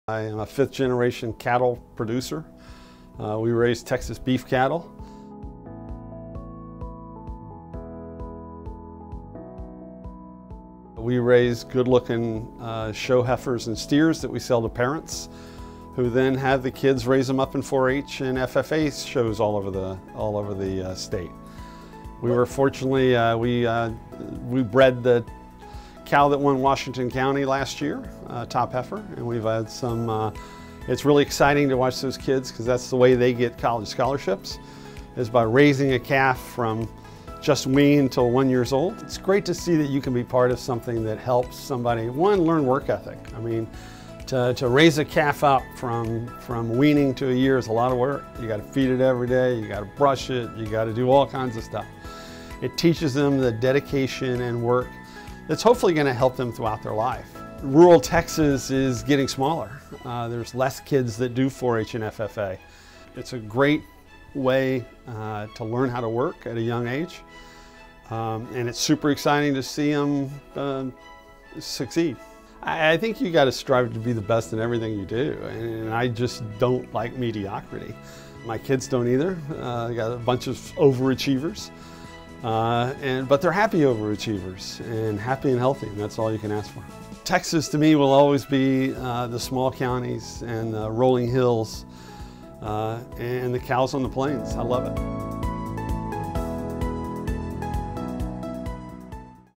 Dr. David Brown on Being a Fifth Generation Cattle Rancher
Did you know that Dr. David Brown is a fifth generation cattle rancher? In this personal video, he shares about raising Texas beef cattle for FFA programs all over the state. Listen to Dr. Brown talk about the benefits of raising a calf and how it instills hard work and dedication that will help kids throughout their life.